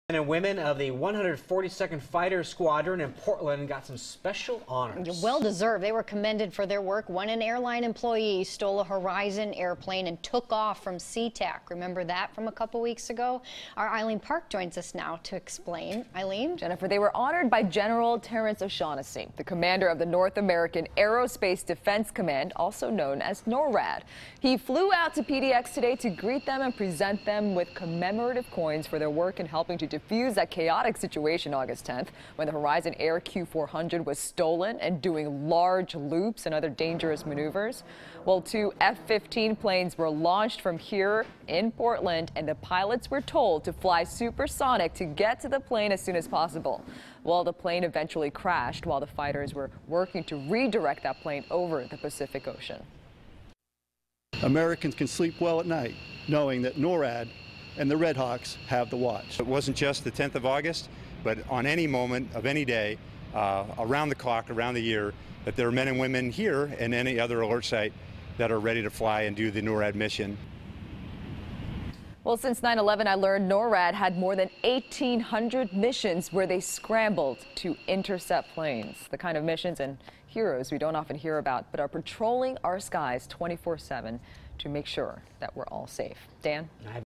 F-15 pilots honored for stolen Horizon Air response
Gen. Terrence O'Shaughnessy congratulated the 142nd Fighter Wing airmen and women who helped navigate the stolen Horizon plane from Seattle with commemorative coins.